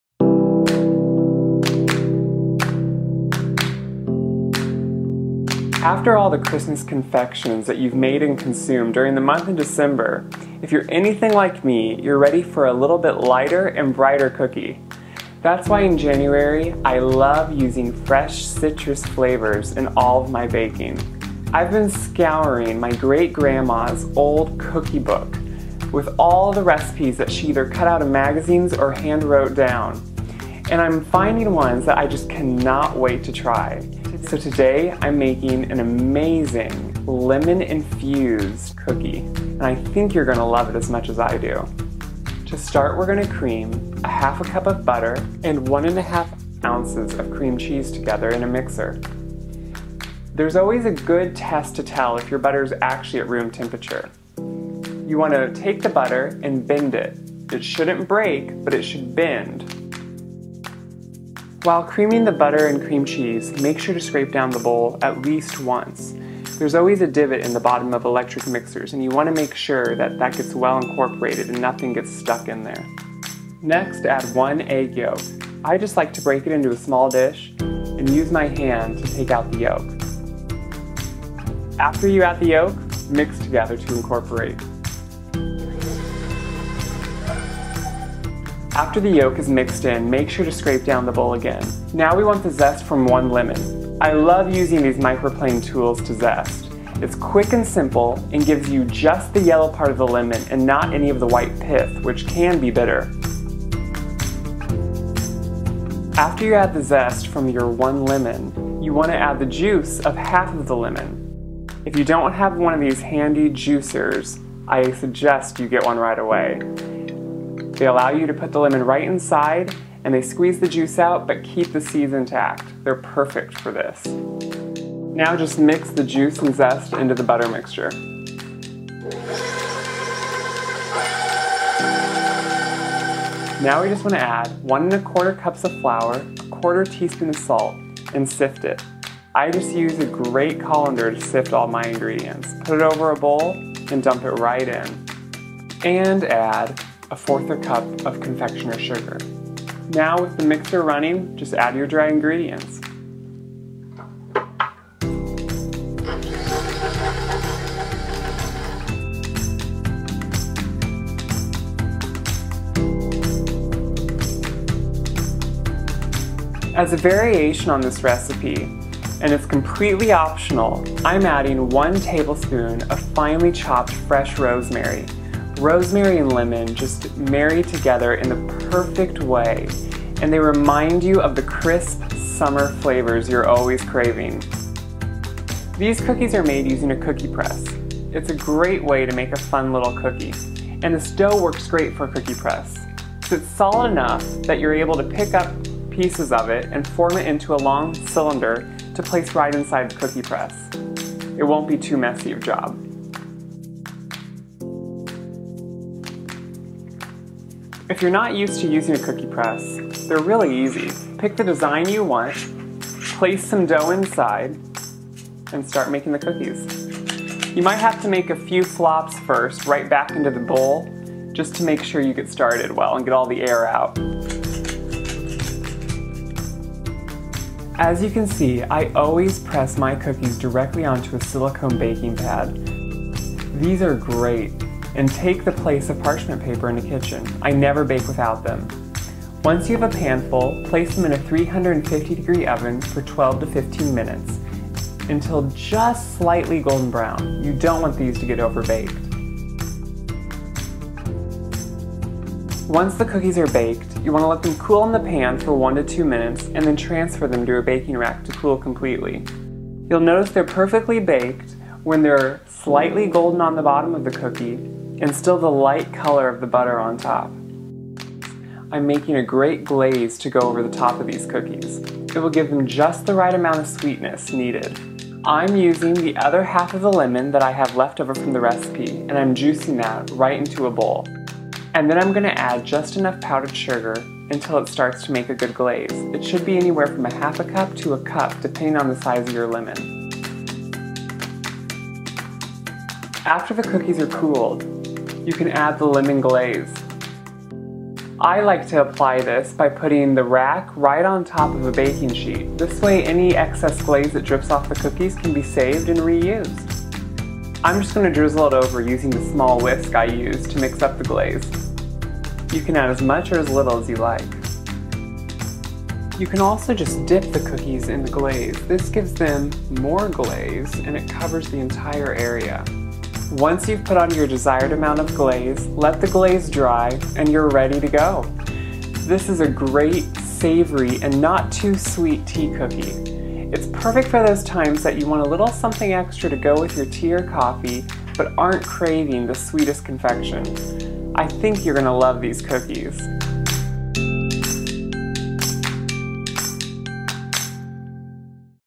Lemon Rosemary Tea Cookies
THE COOKIES

- 1/2 cup butter at room temperature
- 1 1/2 ounces cream cheese at room temperature
- zest from one lemon
- juice of 1/2 lemon
- 1 1/4 cup all-purpose flour
- 1/4 teaspoon salt
- 1/4 cup powdered sugar
- 1 tablespoon minced rosemary (optional)

In an electric mixer, cream butter and cream cheese together until light and fluffy. Add the lemon zest and juice and incorporate it. Sift the dry ingredients and add them to the wet. Mix until the dough comes together around the paddle. Add minced rosemary and mix thoroughly. Using a cookie press, press out any air bubbles back into the mixing bowl and press cookies directly onto a baking sheet. Bake at 350 degrees for 10-12 minutes. The cookies should be lightly golden on the bottom and still light on top.

THE GLAZE

- 1/2 cup to 1 cup powdered sugar

Blend together lemon juice with enough powdered sugar to create a thick glaze. Dip the cookies in the glaze or spread on top of the cookies with a whisk or spoon.